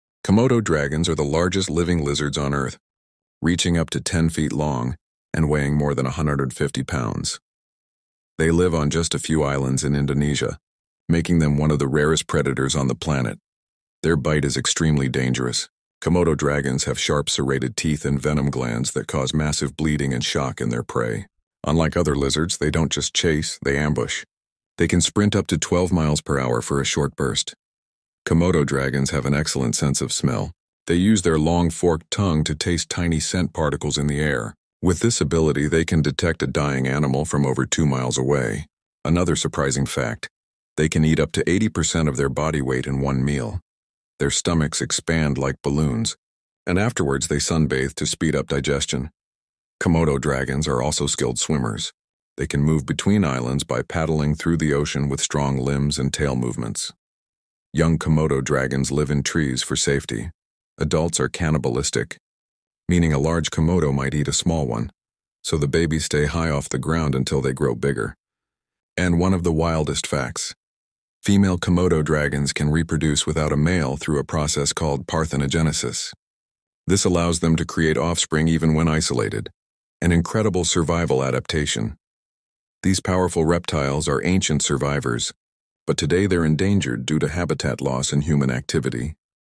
The Lizard That Eats Like a Monster 🦎🤯 | Monitor Lizard Facts
Discover the shocking abilities of monitor lizards — from their super-speed to their massive appetite. These incredible reptiles can eat up to 80% of their body weight and sprint faster than most people expect!